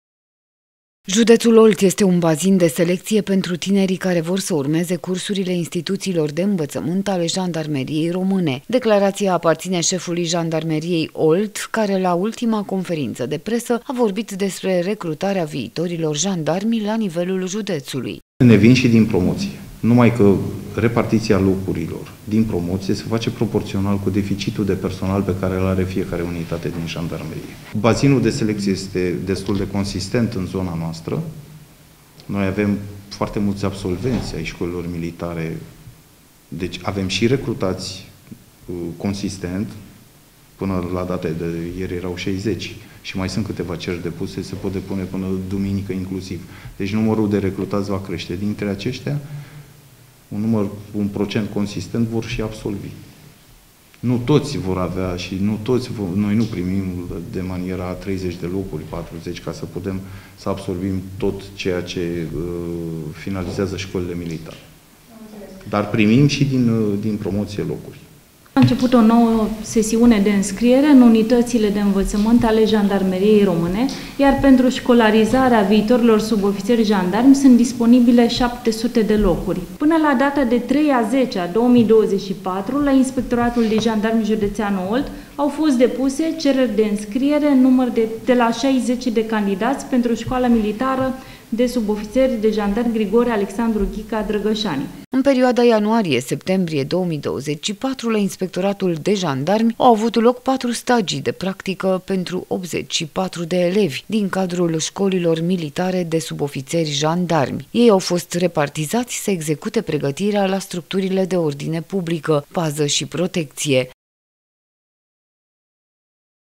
IJJ OLT RECRUTEAZA TINERI PENTRU SCOLILE MILITARE 1
Jandarmeria Olt recrutează tineri pentru școlile militare de subofițeri jandarmi. Zeci de tineri interesați să îmbrace uniforma blue-jandarm au depus deja cererile de înscriere, sperând să obțină un loc în cariera mult visată. Anual, Jandarmeria Olt primește elevi în practică, pe care îi învață cum să acționeze în misiunile la care participă.